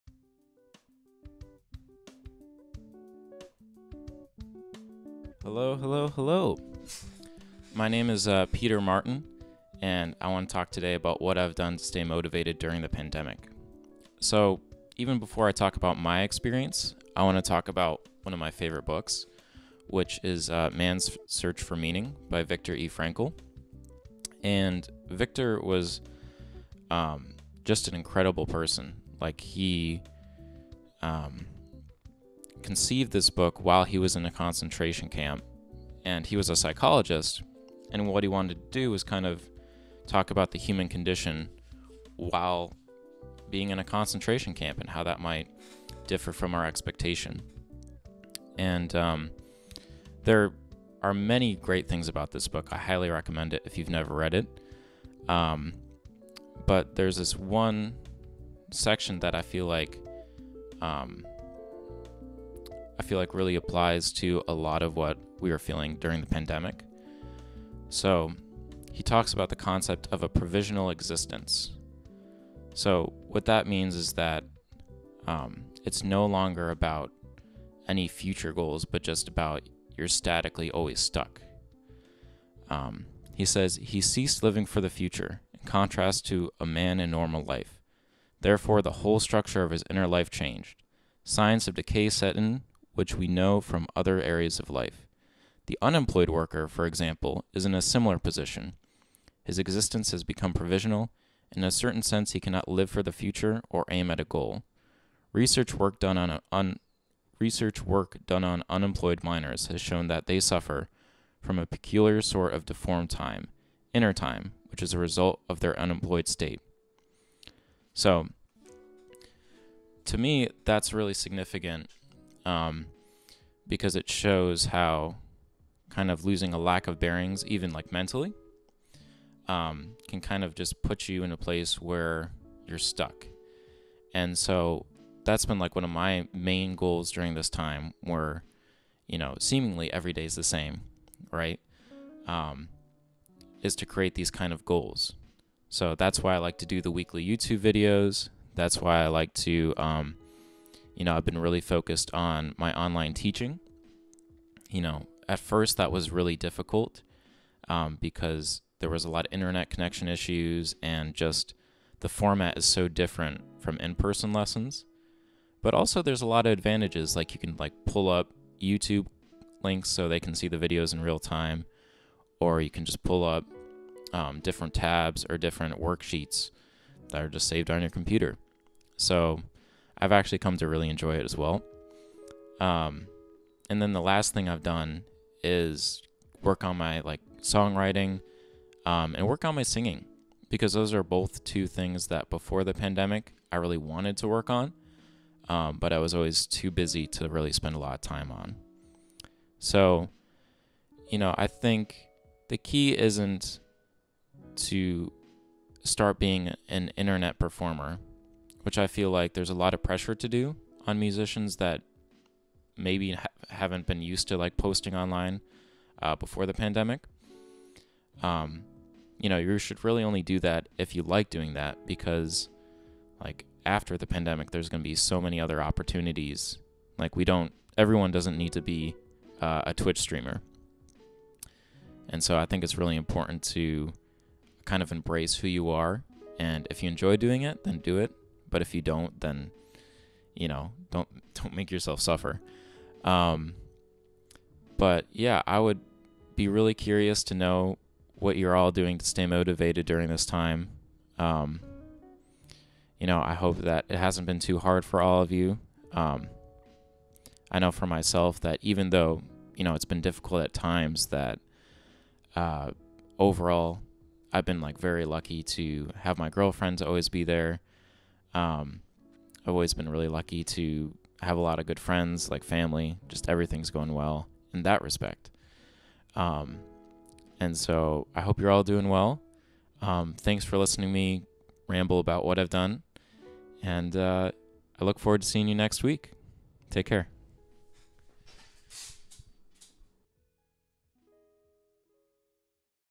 Staying Motivated
I wanted to talk today about how I've stayed motivated during the pandemic. It was really fun to make this "vlog" style video and will definitely be making more in the future.